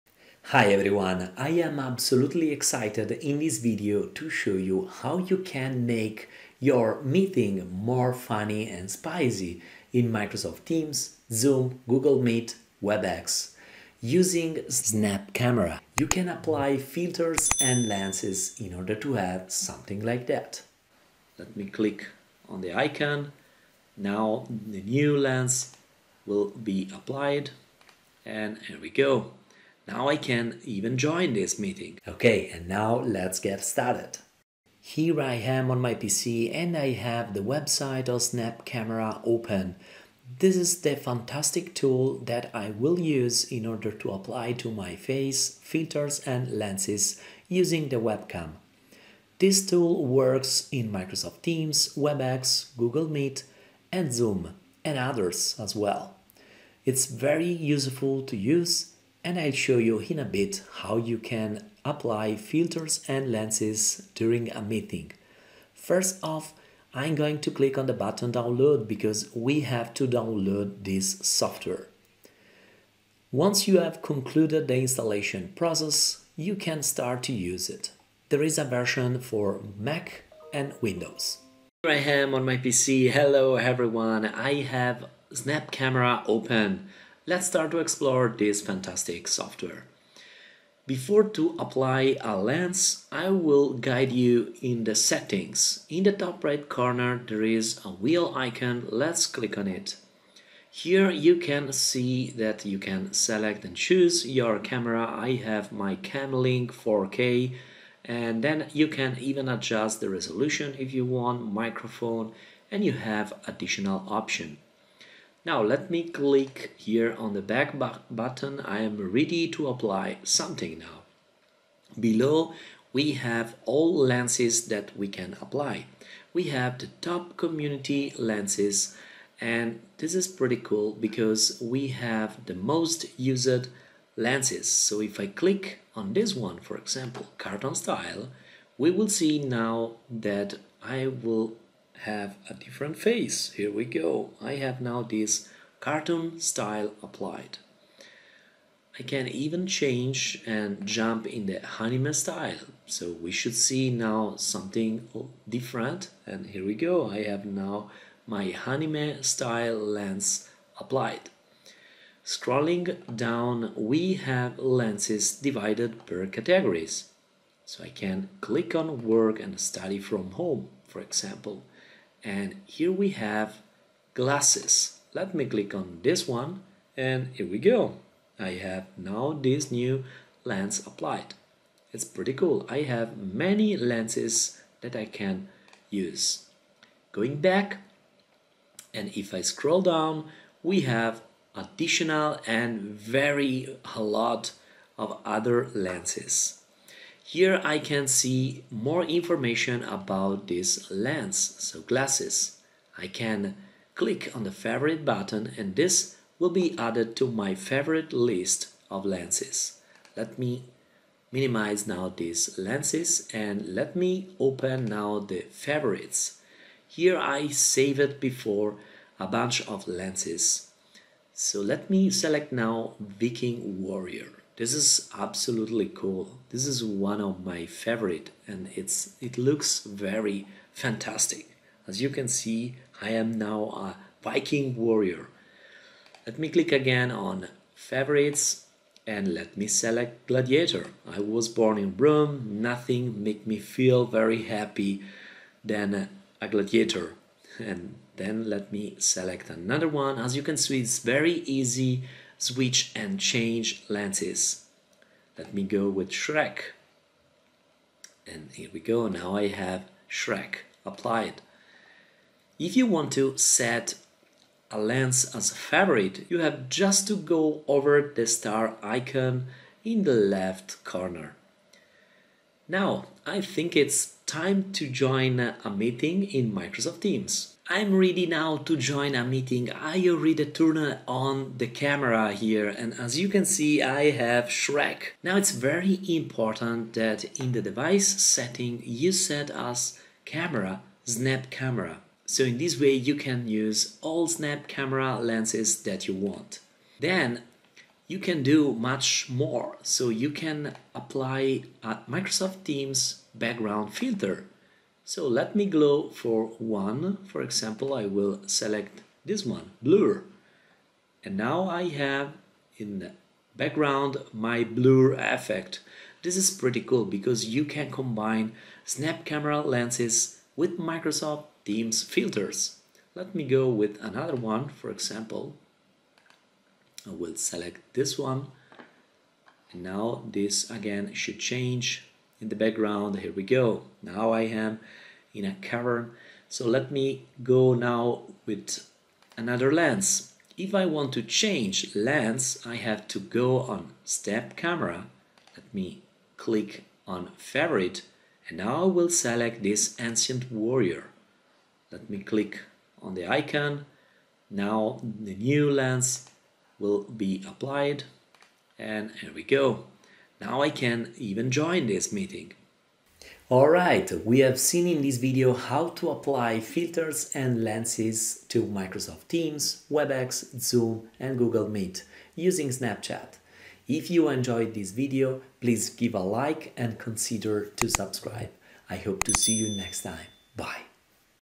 👺How to use filters and lenses in a meeting with Teams, Zoom, Meet, Webex using Snap Camera
In this video tutorial, you’ll learn how to apply filters and lenses in Microsoft Teams using Snap Camera.
I’ll walk you through every step to use lenses in Teams, Zoom, Webex, Google Meet, and others to make your meetings funny.
Snap Camera creates a virtual camera easy to use in Teams, Zoom, Google Meet, Webex, and so on.

⏲Timestamps

00:00 Introduction
00:34 Download and Install Snap Camera

02:08 Snap Camera Quick Tour
04:37 Using lenses and filters in Microsoft Teams
03:39 Conclusion